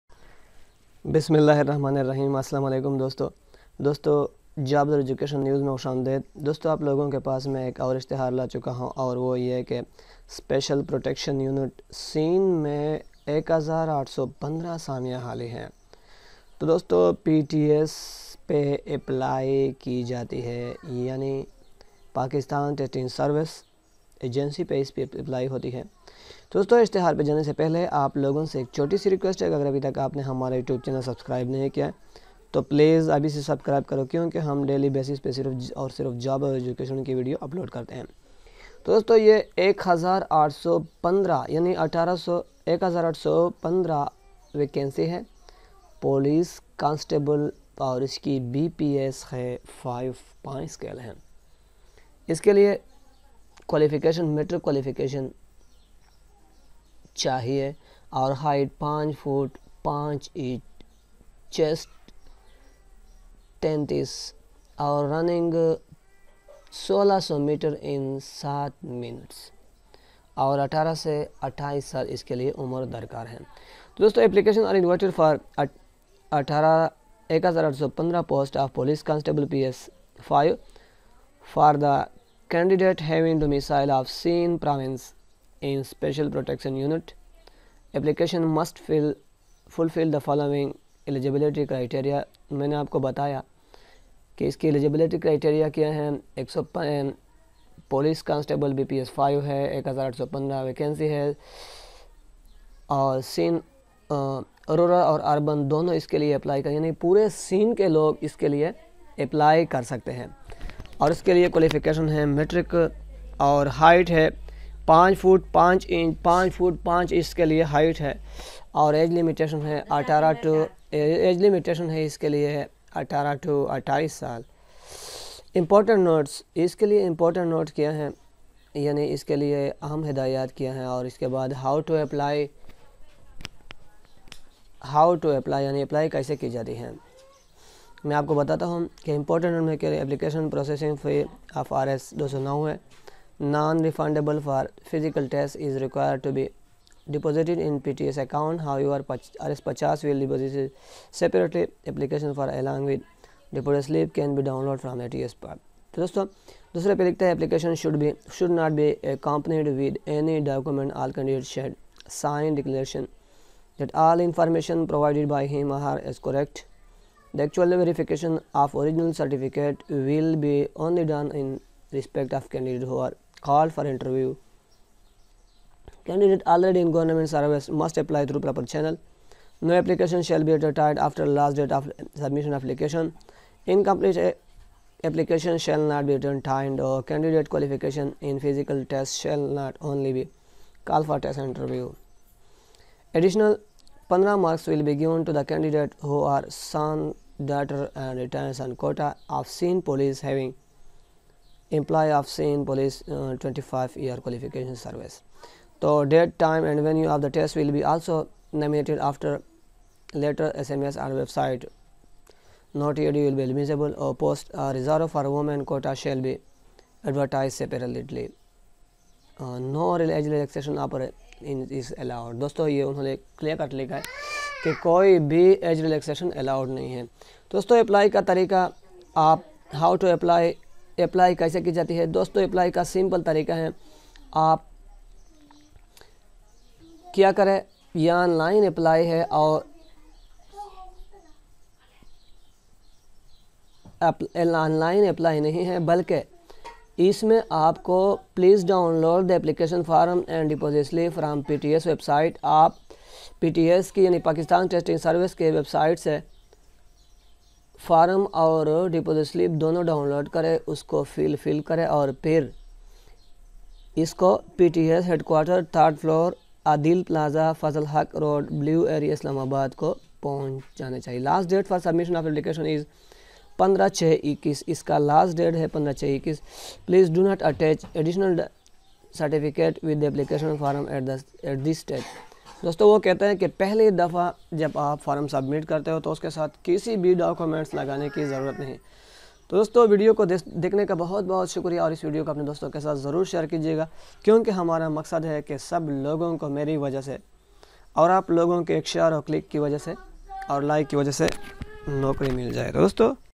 Sind Special Unit police Jobs latest 2021 - 1815 Sind police Jobs 2021
There are 1815 vacancies in Sindh Special Police. Only Sindh Province (Urban and Rural) can apply for this. The age range for this is 18-28 years. Application is made from Pakistan Testing Service Agency.